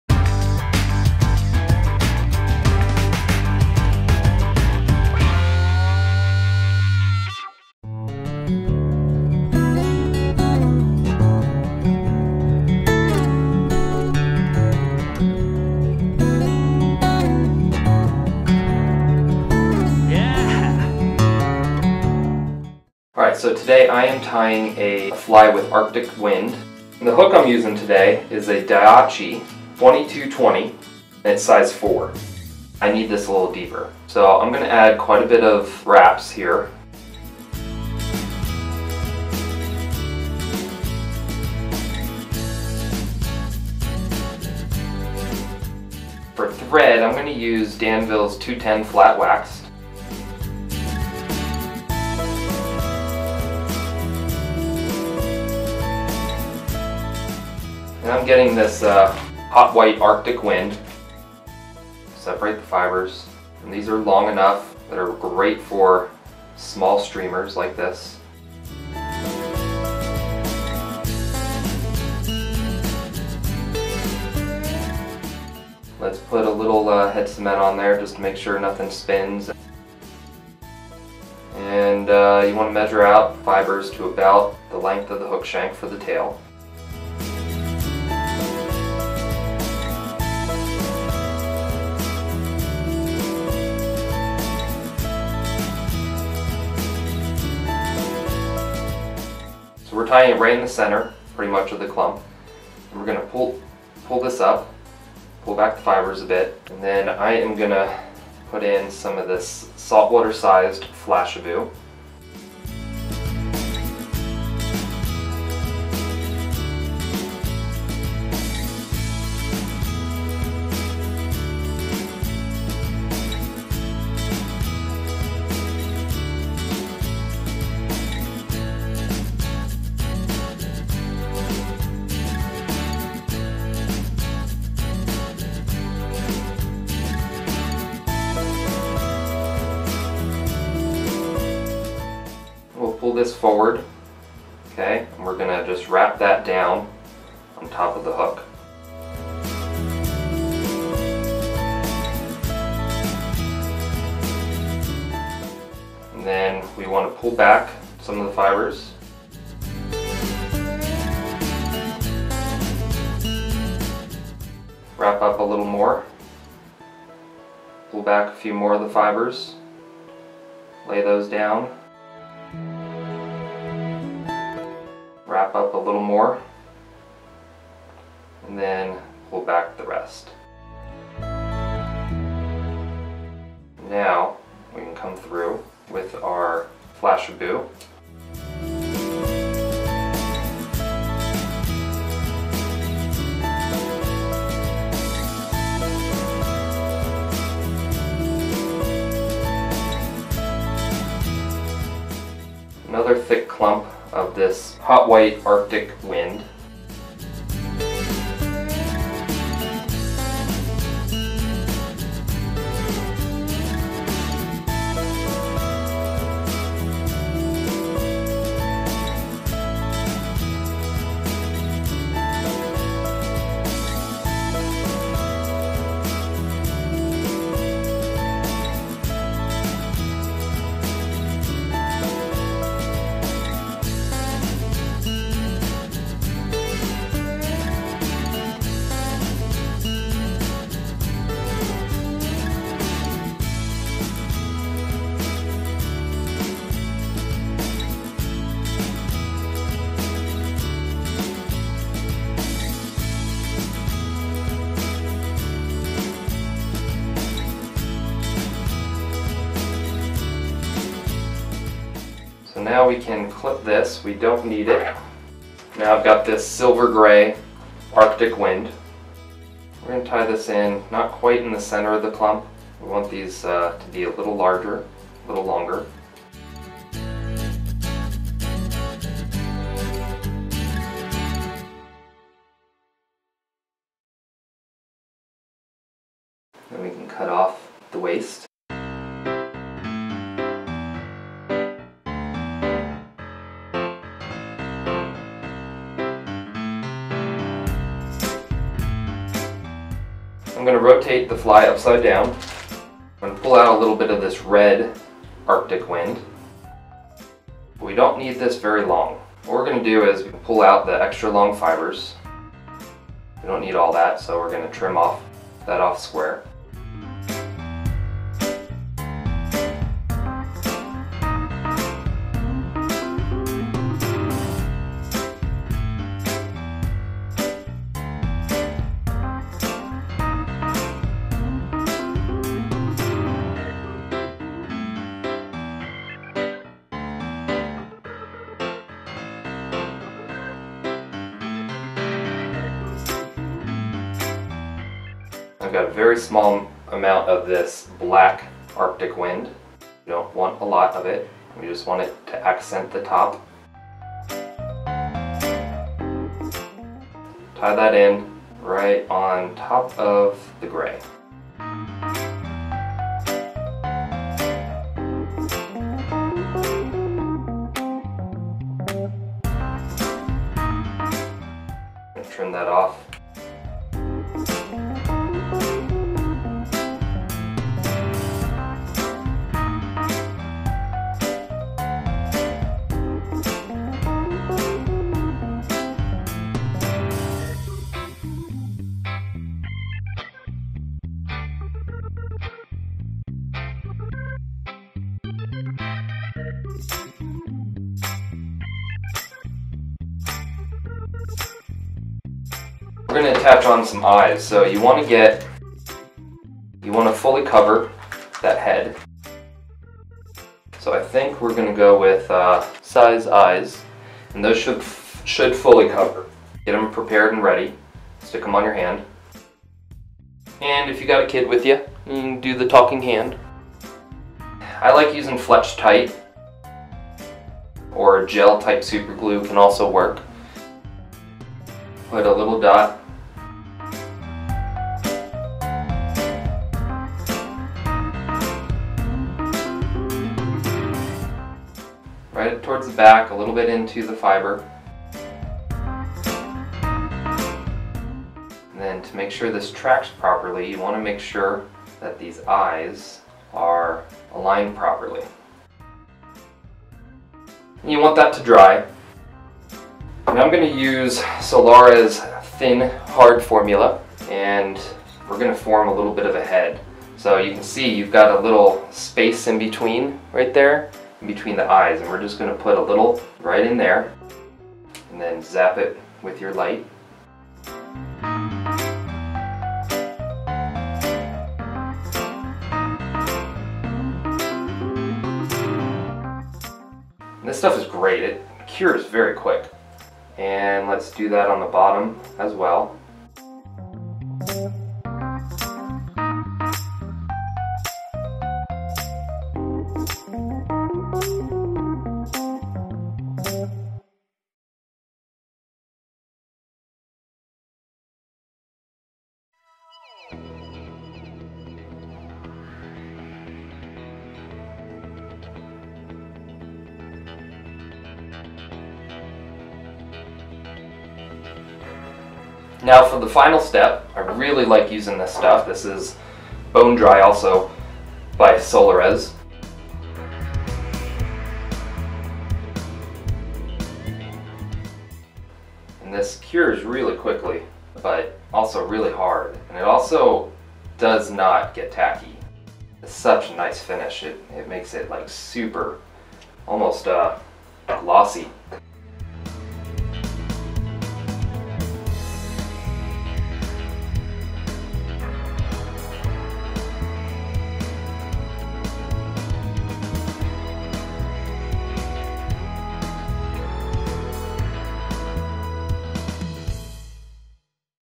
Arctic Wind Glass Minnow - Underwater Footage! - Fly Tying Tutorial
Arctic wind is a new material from Fly Tyers Dungeon that is really nice.  Its a dubbing, with longer fibers that allows you to tie larger flies like glass minnows.  There are a number of colors and its a quality product.  That being said, this fly is very versatile.  With all the colors that FTD offers with Arctic Wind, you should be able to tie any baitfish imitation you want.  It would work great for saltwater flies as well as freshwater flies.  Its a simple tie, and you should be able to tie 5-10 each hour or more.  So you can fill a box quickly.

Hook: Daiichi 2220 - Size 4
Weight: Lead wire
Eyes: 8mm 3D stick on eyes - silver
Adhesive: Fletch tite

The vice I am using is an Anvil Atlas.  Its a full rotary vice, made solid like a tank.  And its very reasonably priced.